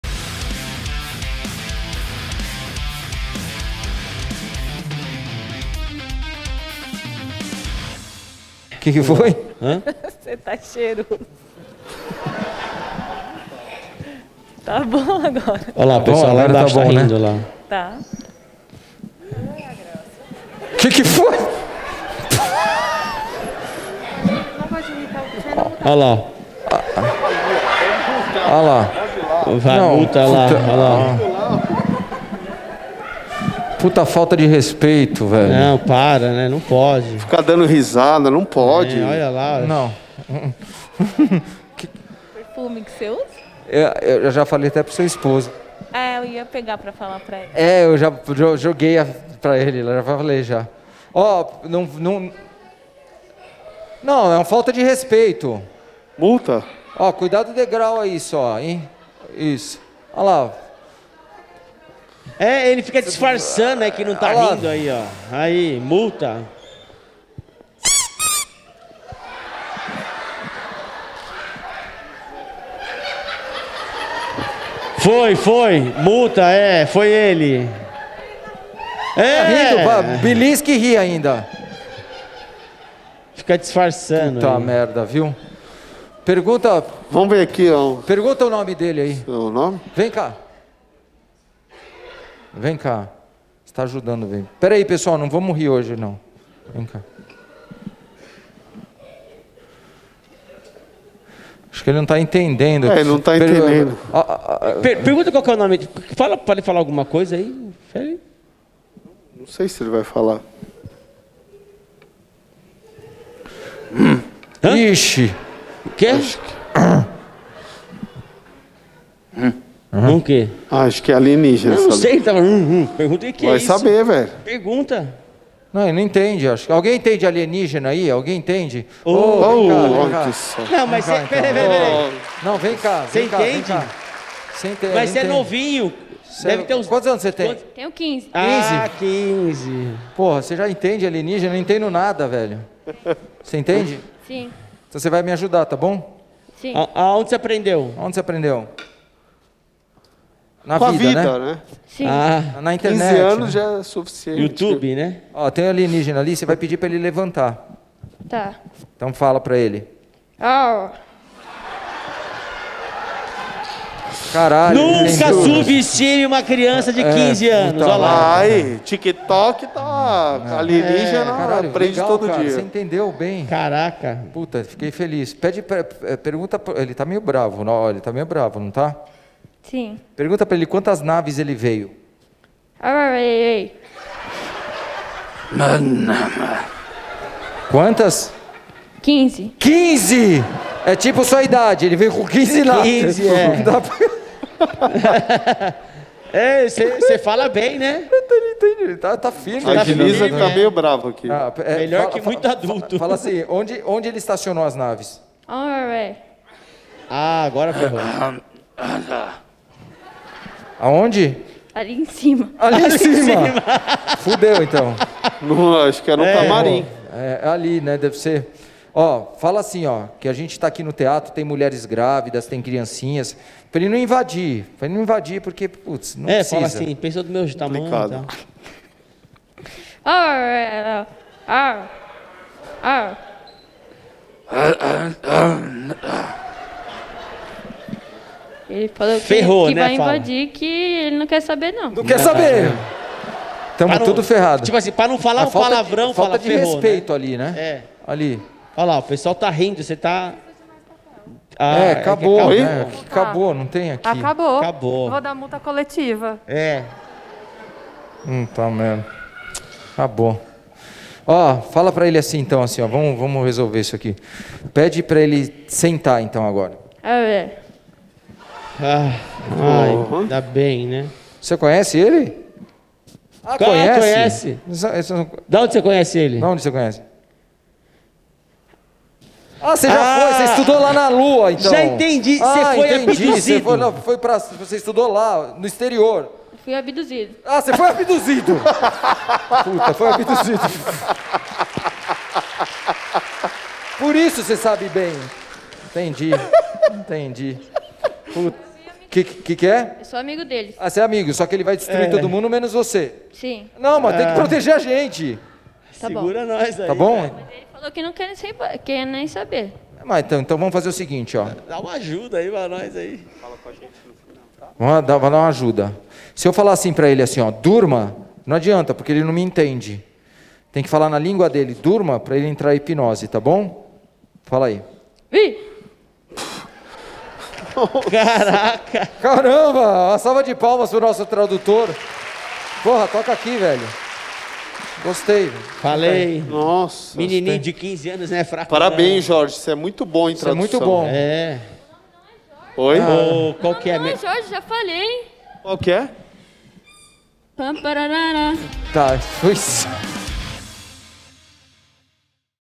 SHOW DE HIPNOSE MAGICAMENTE - JOVEM QUE ENTENDIA O ALIENÍGENA - SHOW 13/02/22
Compartilhe!!!